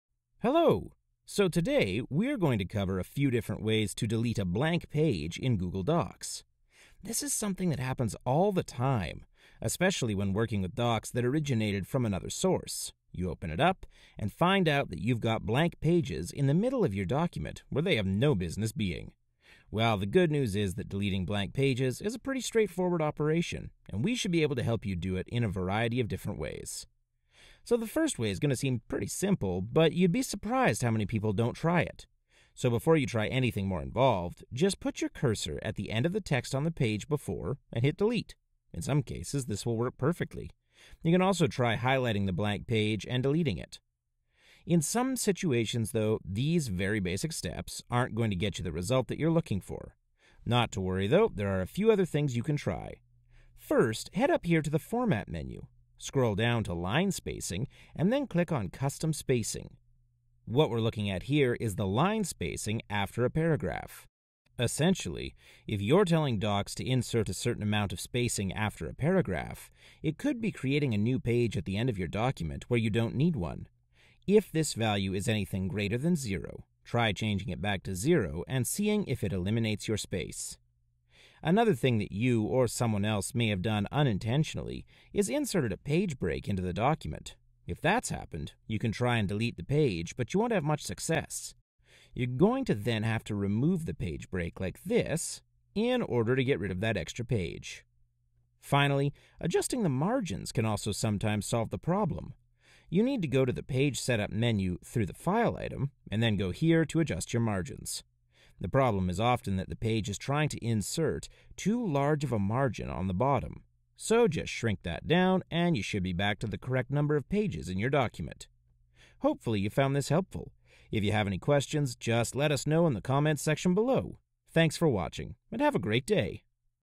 How to Delete an Unwanted Page in Google Docs
Finding unwanted blank pages in your Google Docs happens sometimes, especially when working with documents that originated from another source. The good news is that deleting blank pages in Docs is pretty straightforward, and there a few different ways to do it.